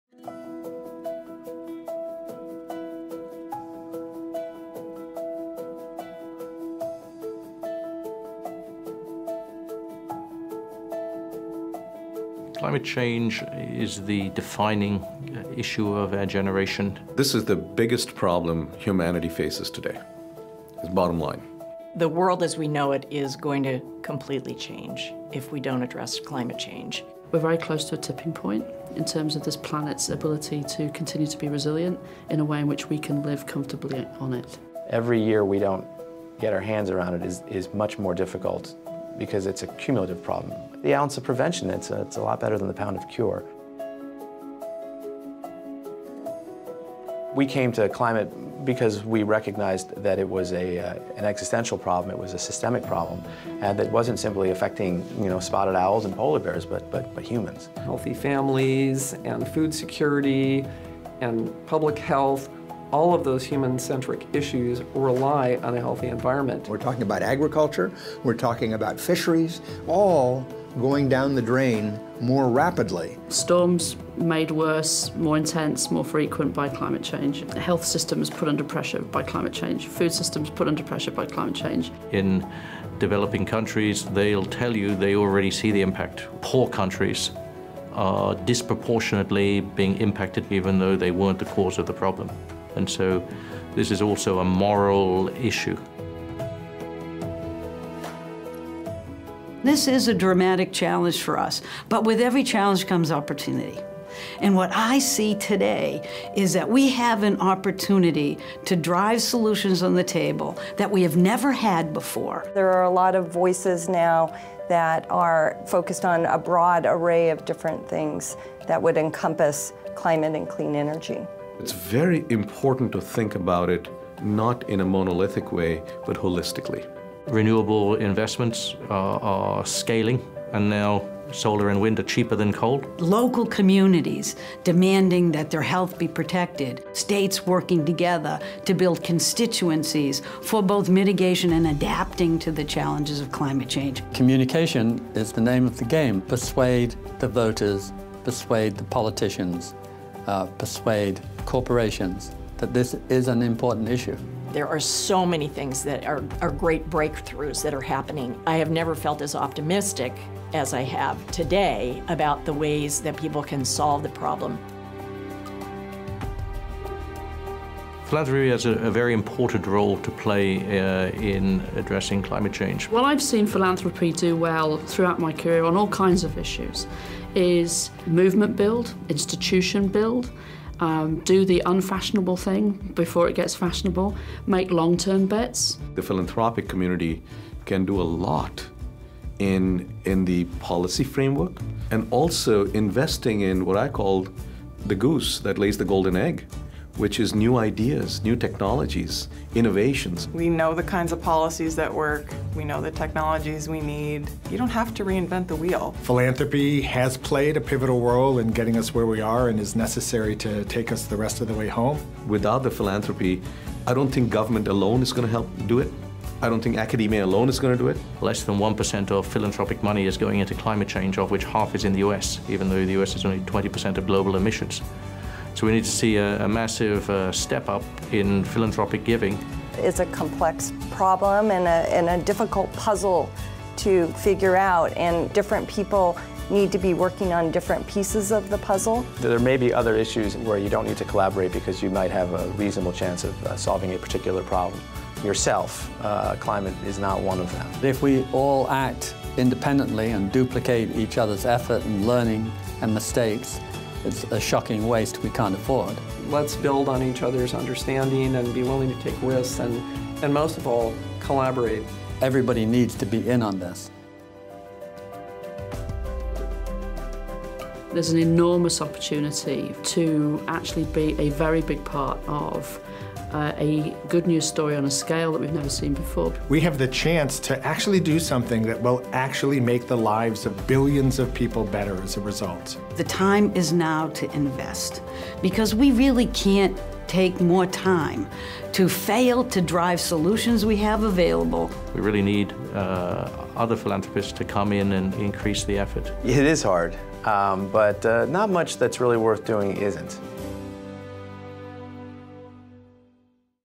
Climate and Clean Energy The Role of Philanthropy in Powering Our Future
Co-hosted by Chris Hohn, Nat Simons and Laura Baxter-Simons, this session explored a variety of approaches for philanthropists to engage on climate change, including overlap with other philanthropic areas such as: clean energy, oceans, health & air quality, food, and forests.